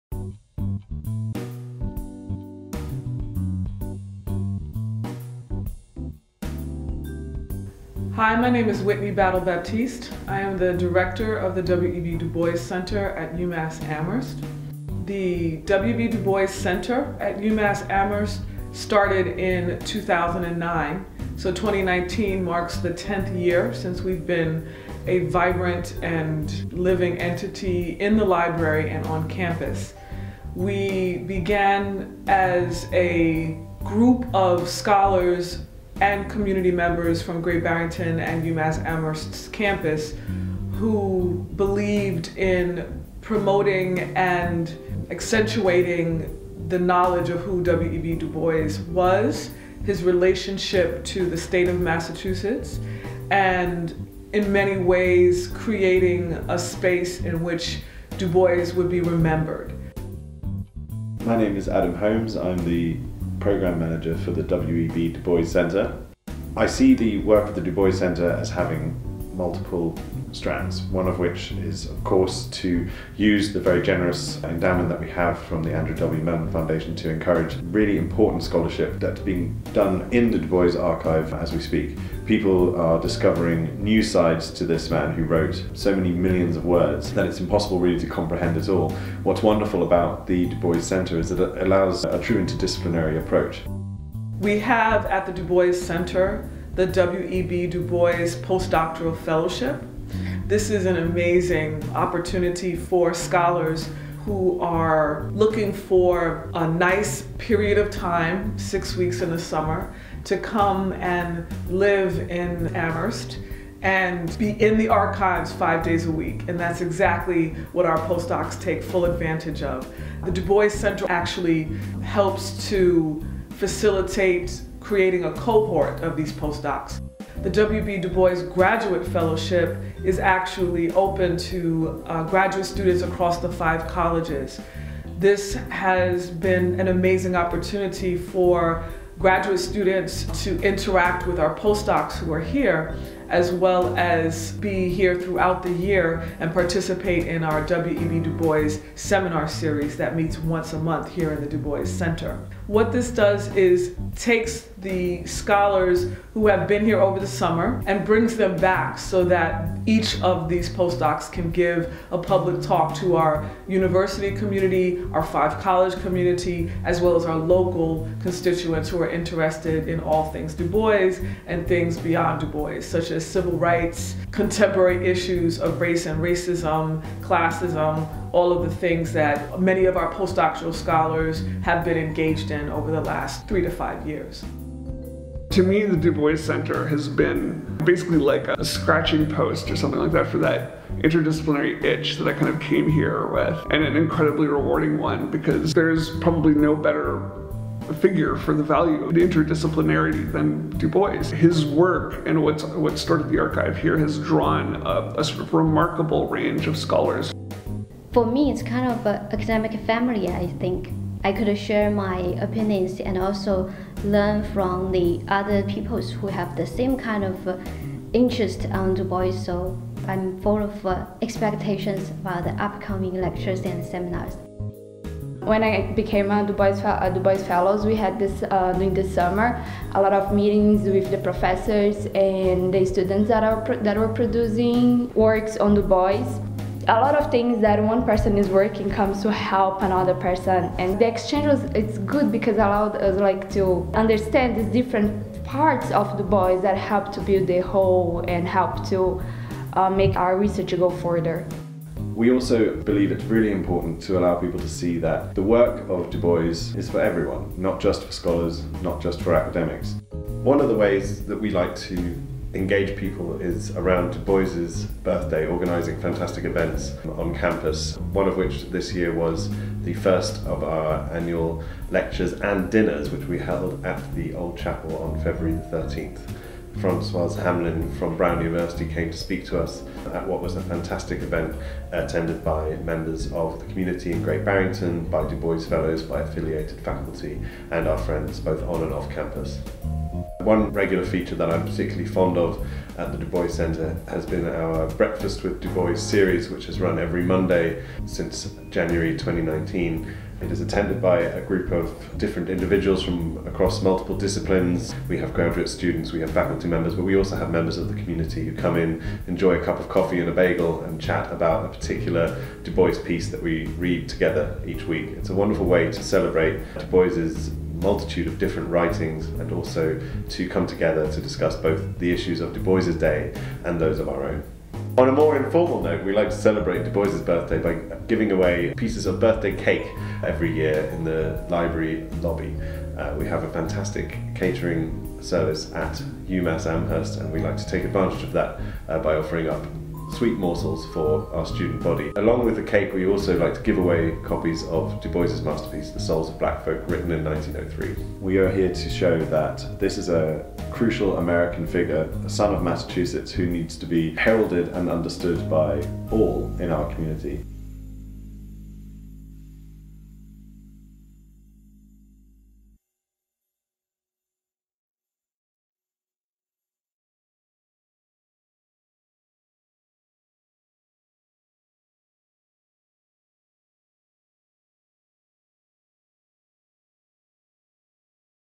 The W. E. B. Du Bois Center
The W. E. B. Du Bois Center at the University of Massachusetts Amherst was established in 2009 to engage the nation and the world in discussion and scholarship about the global issues involving race, labor and social justice. The Center presents an interdisciplinary approach to the intersections among African American culture and history, social justice and labor relations which opens this research to new insights and evaluation in light of the issues confronting people throughout the world today.

By making its resources readily available and accessible to the public, the Center holds fast to the scholarly tradition and spirit of its namesake, W. E. B. Du Bois, a Massachusetts native son, who was pivotal to the social and political debates on race, class and culture of the 20th century.

The Center continues this tradition in four areas: the Fellows Program; the W. E. B. Du Bois Papers and subsequent scholarship; Educational Outreach; and the W. E. B. Du Bois Homesite, of which UMass Amherst is the steward.

The Du Bois Center’s mission is to make the writings of W. E. B. Du Bois and his contemporaries available and accessible to everyone throughout the world for application to the problems and issues of the 21st Century, and to create new knowledge and support scholarship emanating from the life and teachings of W. E. B. Du Bois. The Center connects students, educators, scholars, and the public through lectures, symposia, scholarships and collaborations, to share the vast intellectual resources associated with W. E. B. Du Bois.

Video produced by:
Adam Holmes and Lauren Weiss
UMass Amherst Libraries